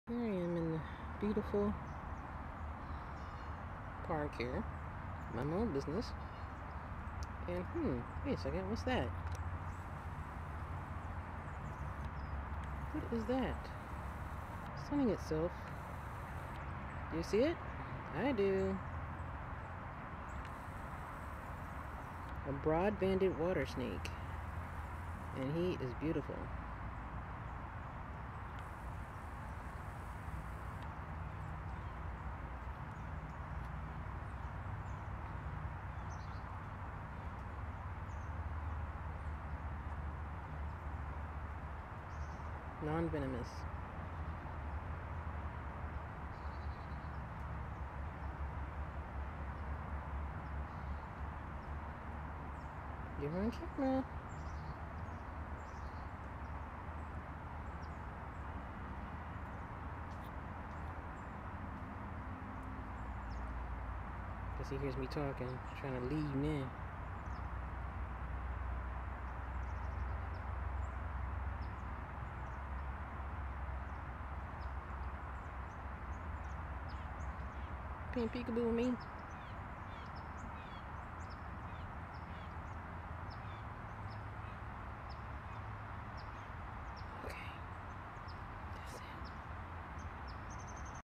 Broad-banded Water Snake pt 1 HD
Property of Kibibi Photography. Broad-banded Water Snake my favorite that I encounter on a frequent basis at the Houston Arboretum and Nature Center.